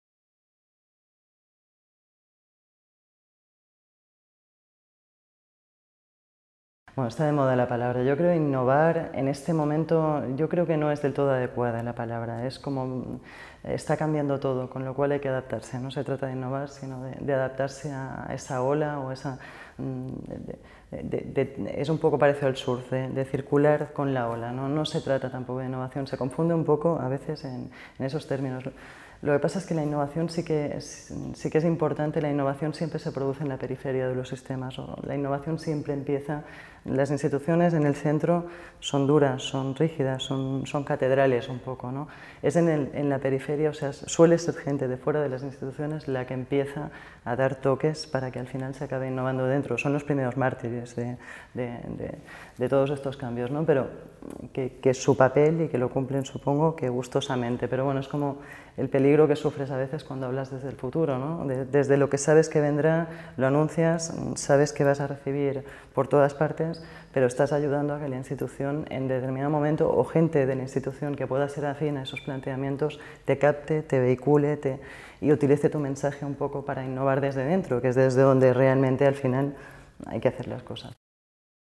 Icionline-Dolors Reig-Innovación
Entrevista en CCCB Lab, ¿Qué es la innovación? Dolors Reig,